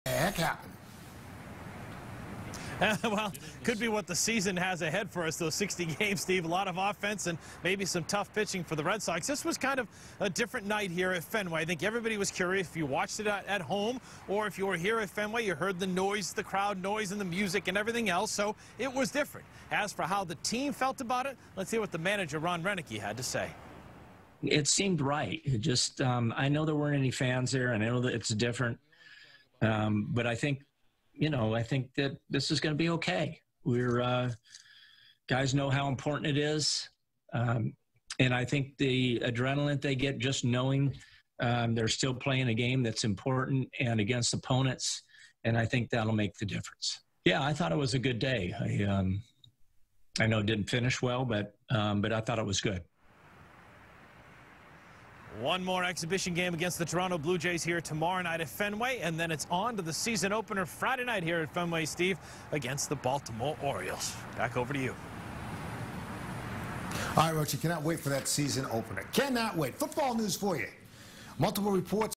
Ron Roenicke On Red Sox Exhibition Game
Ron Roenicke said despite losing the exhibition game against the Blue Jays, it was "a good day."